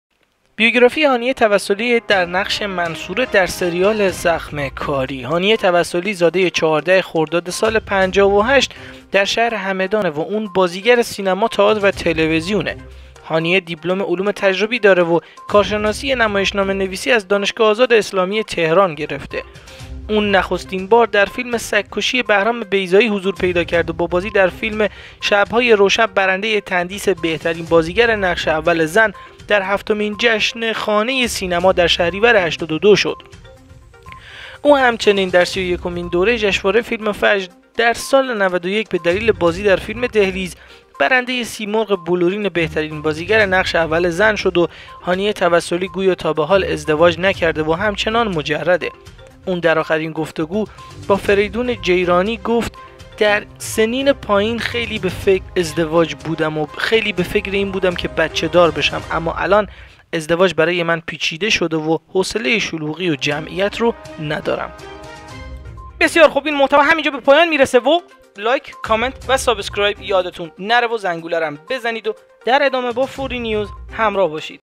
بیوگرافی هانیه توسلی نقش منصوره در سریال زخم کاری - زندگینامه هانیه توسلی
هانیه توسلی زاده 14 خرداد سال 1358 در شهر همدان است او بازیگر سینما، تئاتر و تلویزیون است.
هانیه دیپلم علوم تجربی دارد و کارشناسی نمایشنامه نویسی از دانشگاه آزاد اسلامی تهران را دارد.
او نخستین بار در فیلم سگ کشی بهرام بیضایی حضور پیدا کرد.
با بازی در فیلم شب های روشن برنده تندیس بهترین بازیگر نقش اول زن در هفتمین جشن خانه سینما در شهریور 82 شد. او همچنین در سی و یکمین دوره چشنواره فیلم فجر در سال 91 به دلیل بازی در فیلم دهلیز برنده سیمرغ بلورین بهترین بازیگر نقش اول زن شد.
هانیه توسلی گویا تا به حال ازدواج نکرده است و همچنان مجرد است. او در آخرین گفتگویی که با فریدون جیرانی داشت در این مورد گفت : در سنین پایین خیلی به این فکر می کردم که ازدواج کنم و بچه دار شودم. اما الان ازدواج برای من پیچیده شده است و حوصله شلوغی و جمعیت را ندارم.